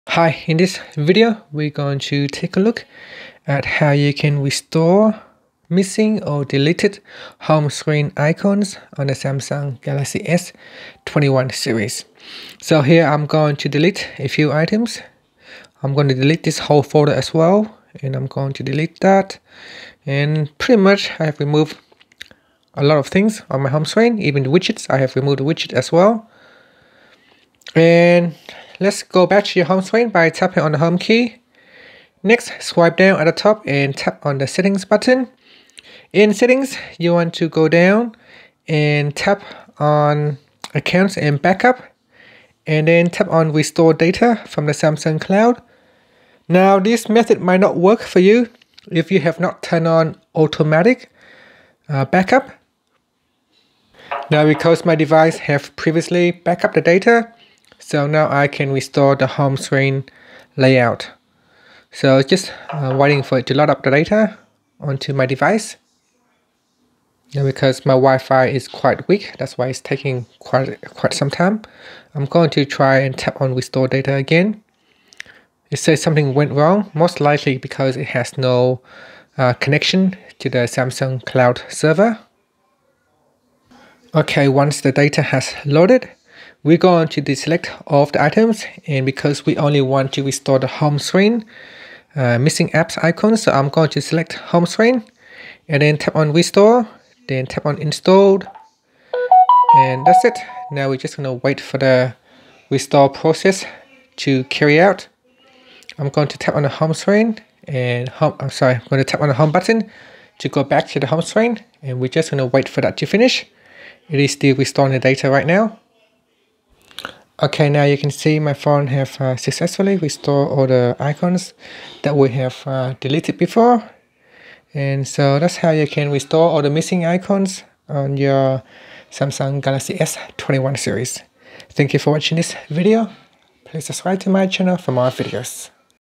Galaxy S21/Ultra/Plus: How to Restore Missing or Deleted Home Screen Icons
Learn how you can restore missing or deleted home screen icons on the Galaxy S21/Ultra/Plus.